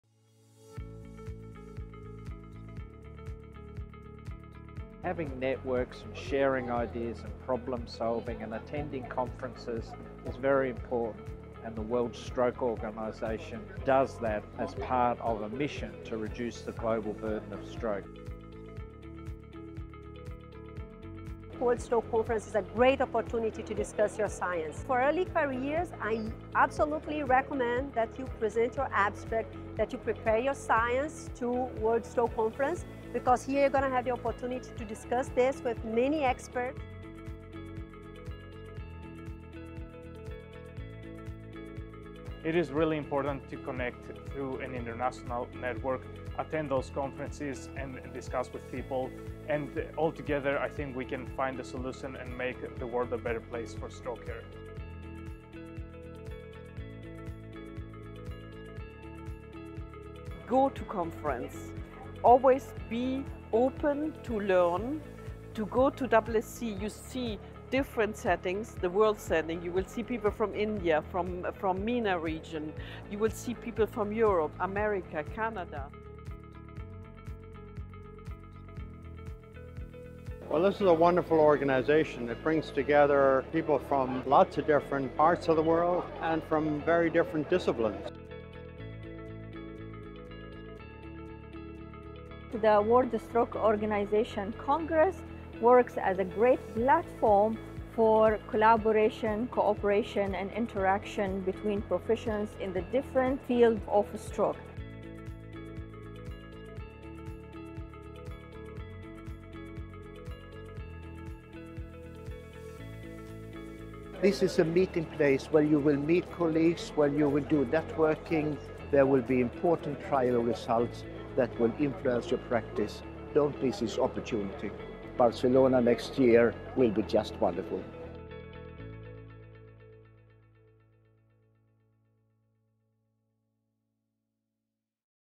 WSC 2025 | ОCTOBER 22–24 | BARCELONA, SPAIN
The World Stroke Congress brings together clinicians, researchers, health policymakers, and stroke advocates from around the globe. The congress delivers cutting-edge science, practical education, and powerful patient perspectives, all under one roof.

The WSC 2025 program is designed to provide you with essential insights and the latest advancements in stroke care, featuring a variety of session formats such as:
- Plenary presentations from global experts
- Joint sessions with renowned organizations
- Roundtable discussions
- Interactive networking and debate sessions, and much more!

Key topics include:
- Innovations in stroke care technology
- Managing vascular cognitive impairment
- WSO & SIECV: Integrating Technology to Enhance Stroke Care and Education
- Medical Journals Roundtable: What editors-in-chief are seeking
- Debate session: The future of neurocardiology